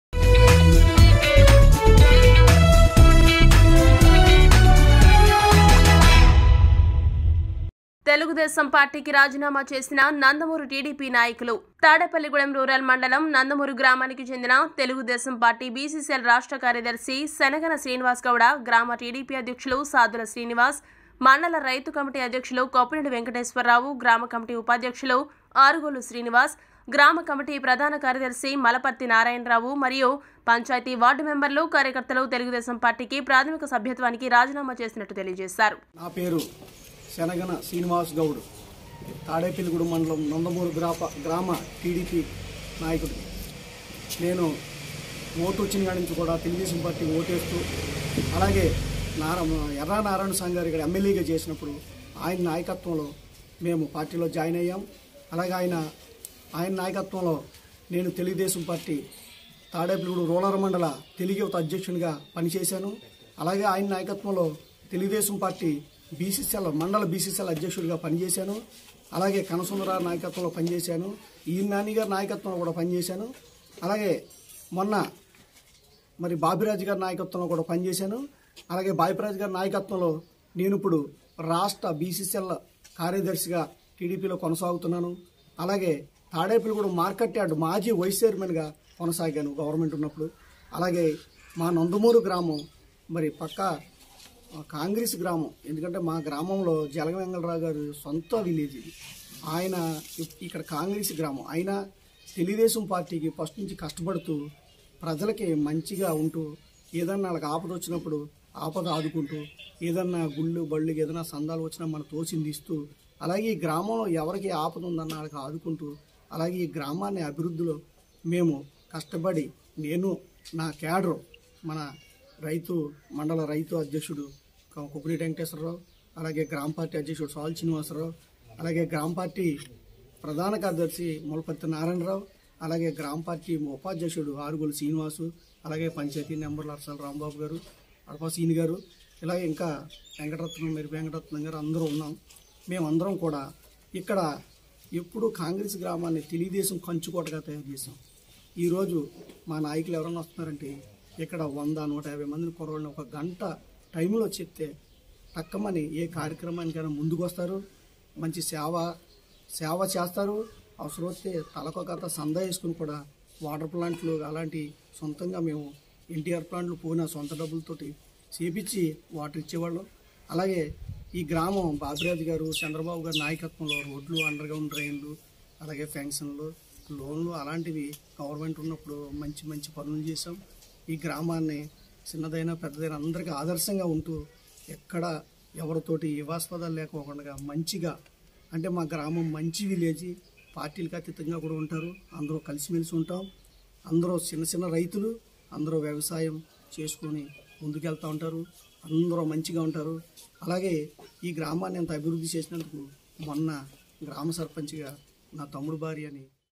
తాడేపల్లిగూడెం నందమూరు గ్రామానికి చెందిన టిడిపినాయకులు రాజీనామా
@truthnewstelugu తాడేపల్లిగూడెం నందమూరు గ్రామానికి చెందిన టిడిపినాయకులు రాజీనామా